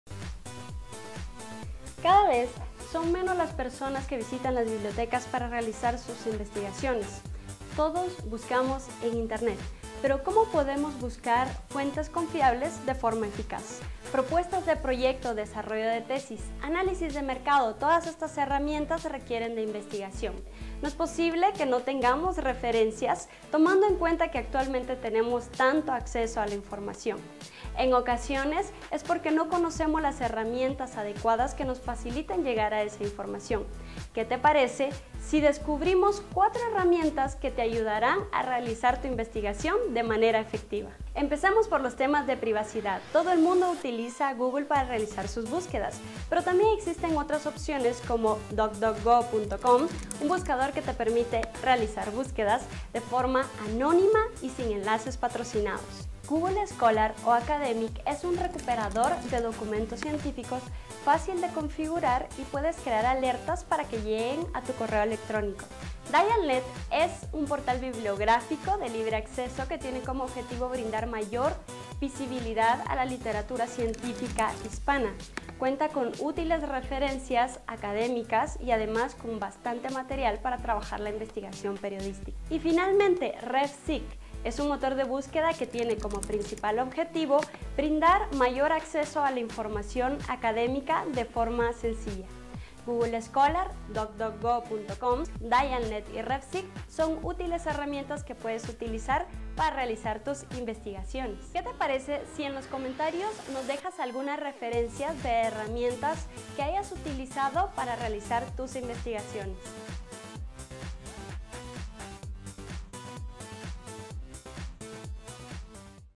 ¿Cómo investigar de manera confiable en la web?
En Internet encontramos muchas fuentes de información pero en ocasiones necesitamos referencias específicas que nos ayuden a trabajar en nuestra tesis de grado o cierto tipo de investigación.

En esta ocasión @stephaniefalla comparte 4 herramientas que filtran el contenido y ofrecen resultados a sitios con documentos académicos confiables y útiles para recopilar información de manera efectiva.